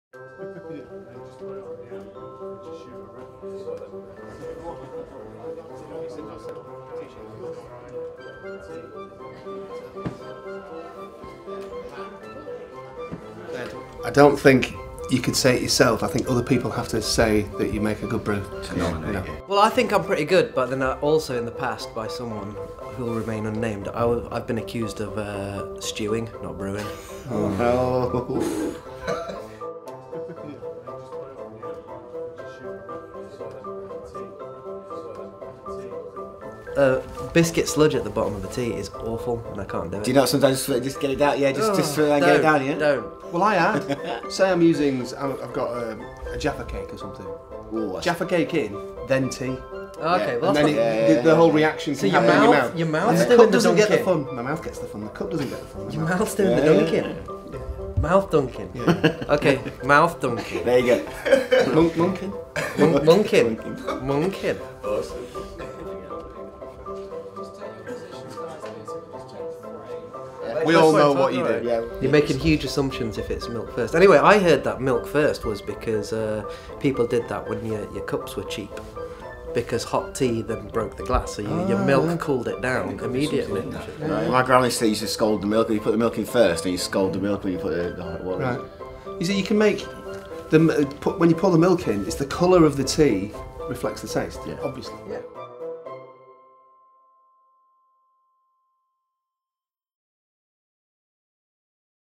Kaiser Chiefs Talk Tea
When they're not hanging around in Yorkshire Tea's reception playing hold music for phone callers, the Kaiser Chiefs like to debate the finer points of brewing up and dunking.